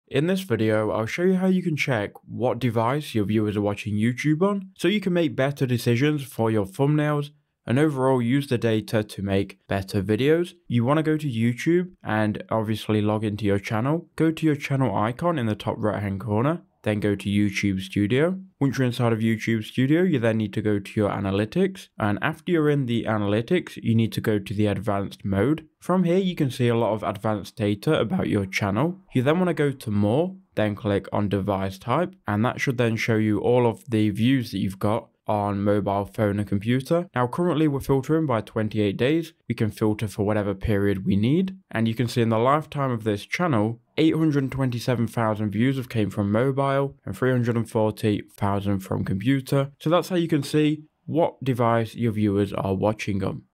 How To See What Device Your Viewers Are Watching YouTube On (Step By Step)
In this video, I show you how to see what device your viewers are watching YouTube on. Did this video help? Thanks!